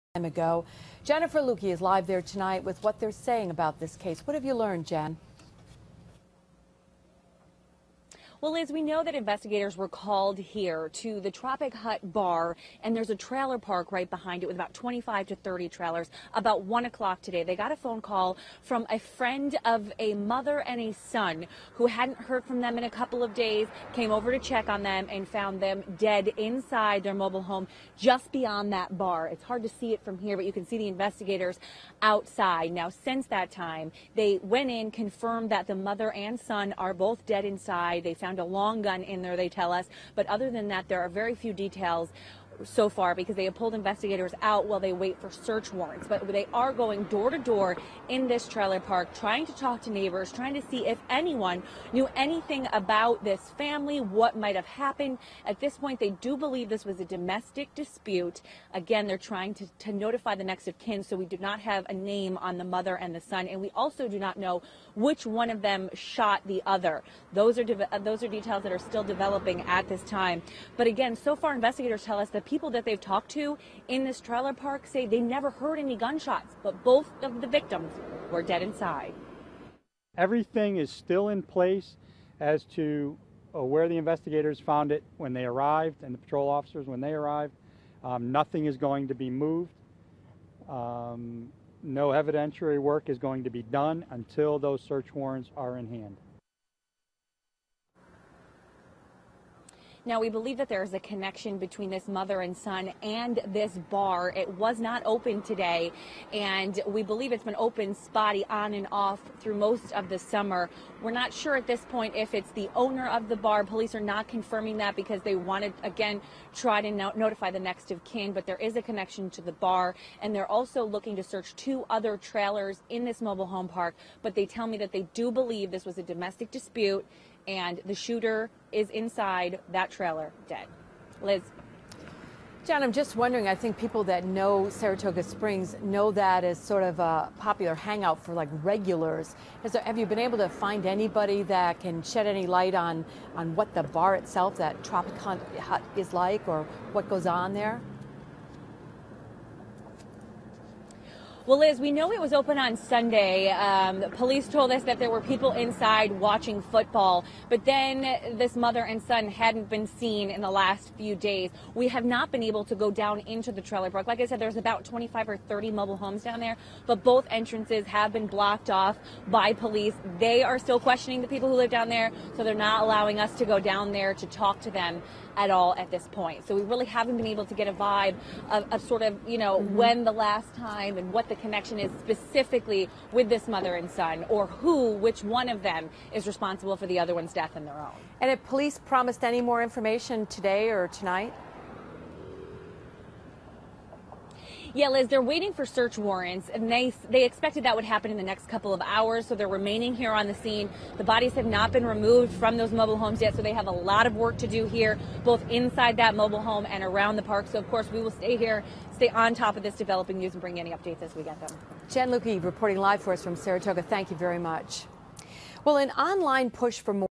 UPDATE: Mother and son found dead in Saratoga Springs
SARATOGA SPRINGS -- After autopsies were conducted Saturday, police announced the crime in Saratoga Springs was a murder-suicide.The bodies of Patricia Ambrozak, 75, and John Ambrozak, 47, were found Friday afternoon in their trailer near the north end of the 9P bridge over Saratoga Lake. According to police, in the early morning hours of September 30th, John Ambrozak assaulted his mother, broke several of her bones and stabbed her to death. Police say he remained in the mobile home with his mother's body for twelve to fourteen hours until late afternoon, then took his own life in the mobile home with a single gunshot. According to police, records show a prior domestic incident in 2011 where John Ambrozak was arrested for assaulting his mother.SARATOGA SPRINGS - Police are investigating the discovery of two bodies found in a home near the lake off Route 9P.Saratoga Springs Police say the victims were mother and son, both were found shot with a long gun.When they hadn't been seen for a few days, a family friend discovered them and called police.Police are questioning residents.CBS6 has a crew on the scene.